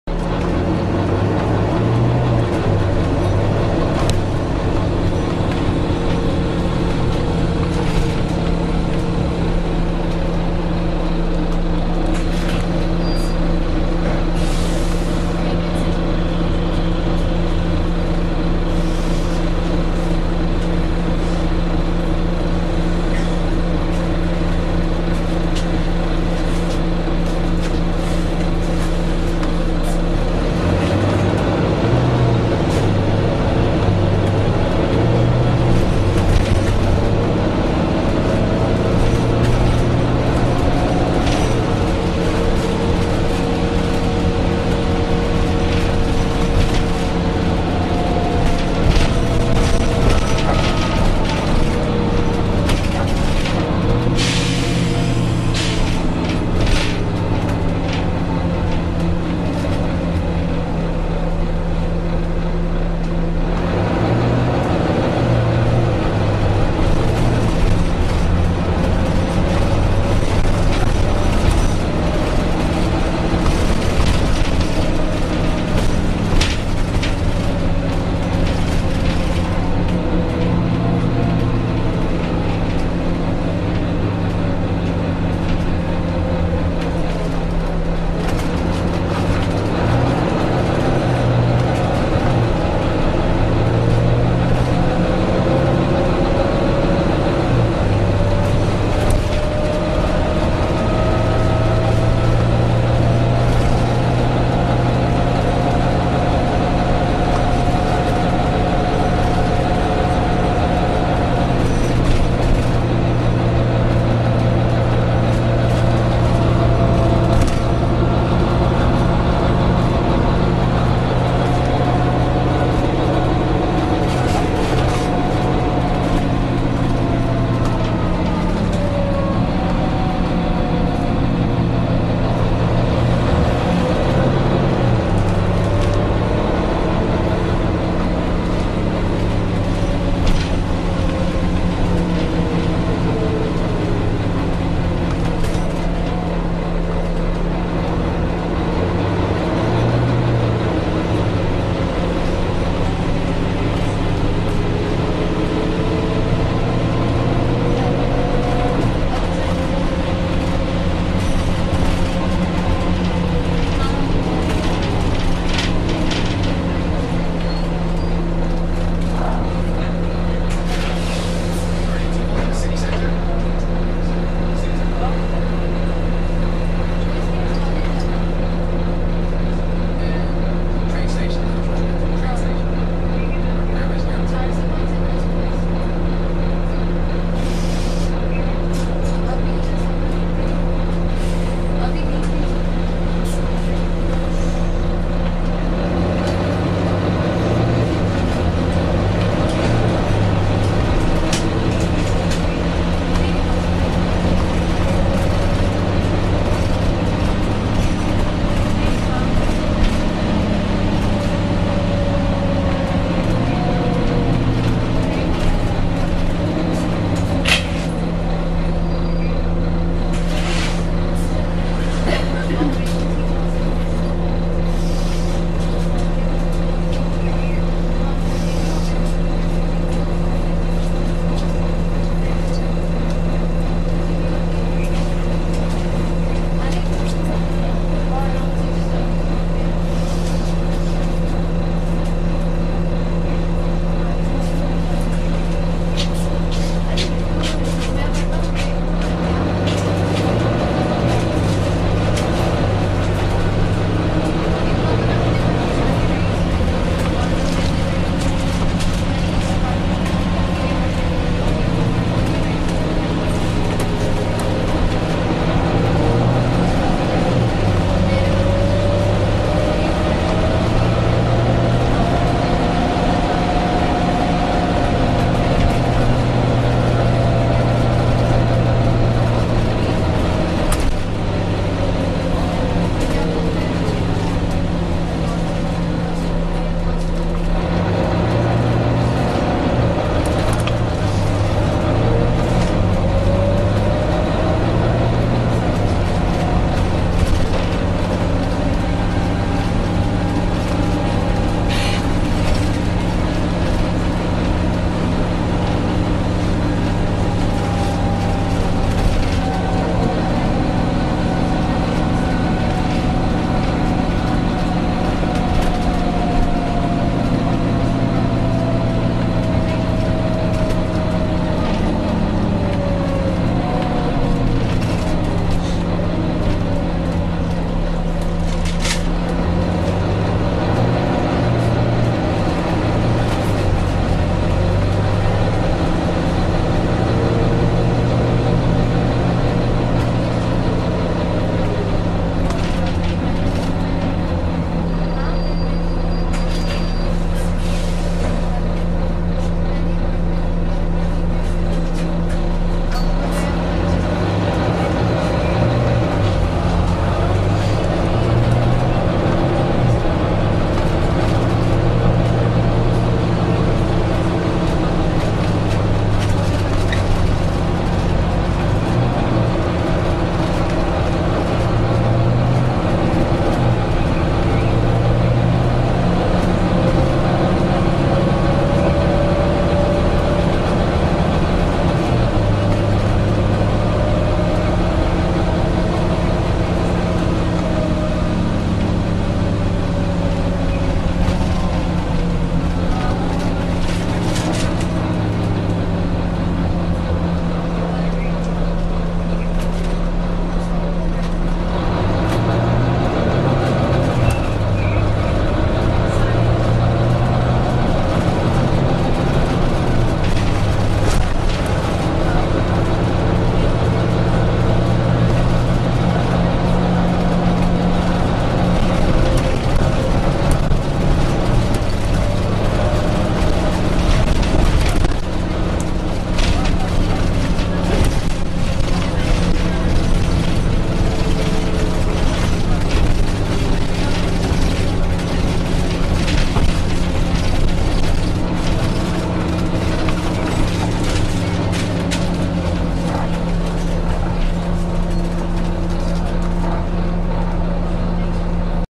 First WY Wright Gemini 37692 (YJ09 OAG)
Taken on 37692 (YJ09 OAG) from Moor Allerton Centre to Leeds on service 51.